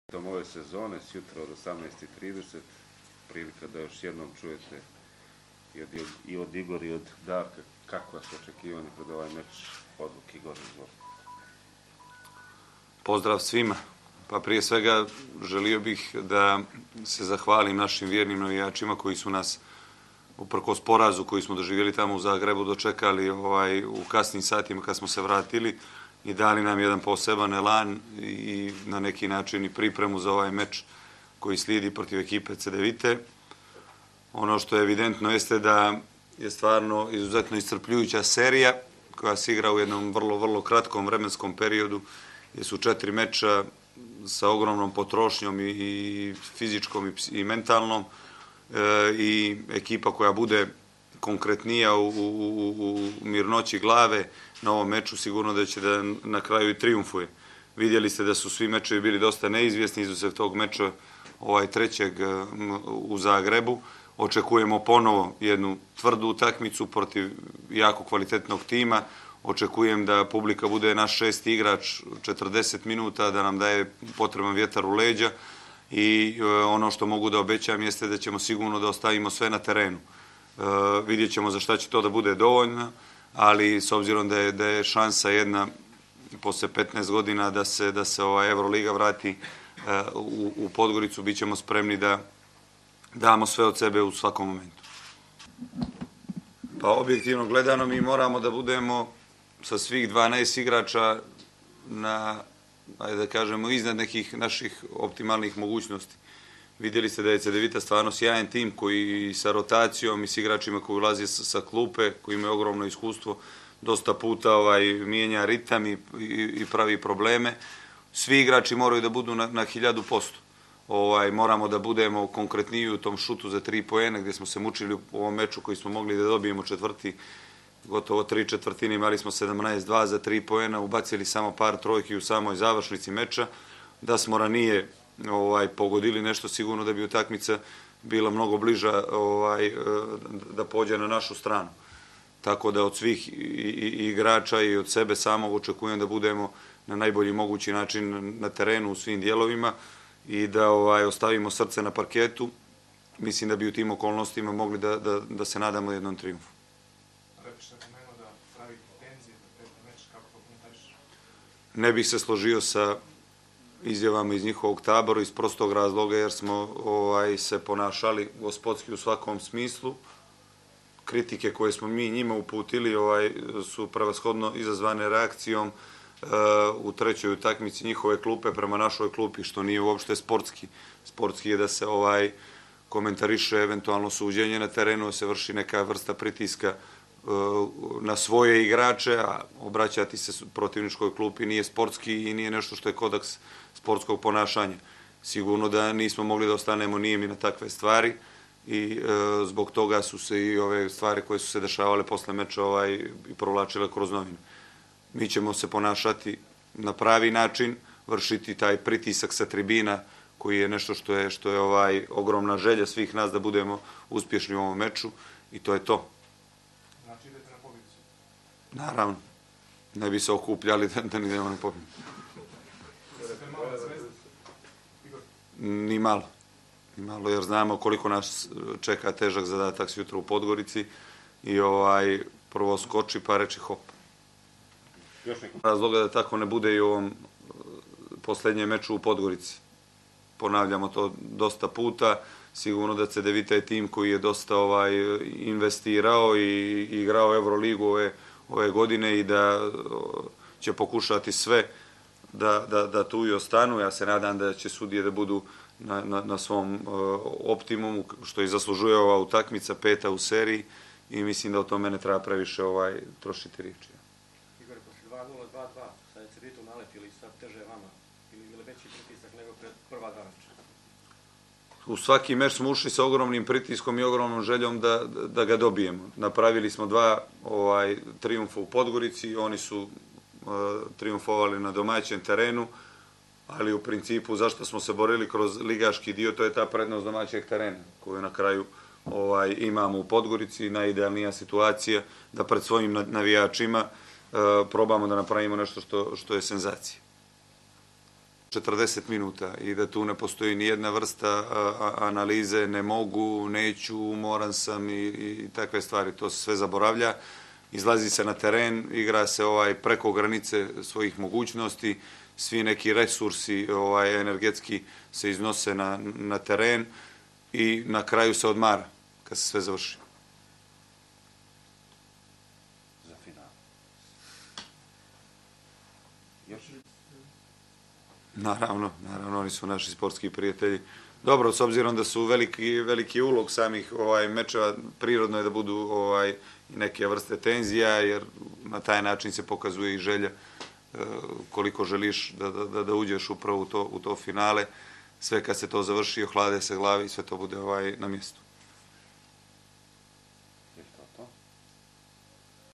Igor Jovović before the 5th semi-final game
Budućnost VOLI head coach Igor Jovović spoke about his expectations before the beginning of the fifth game of the semi-final series against Cedevita.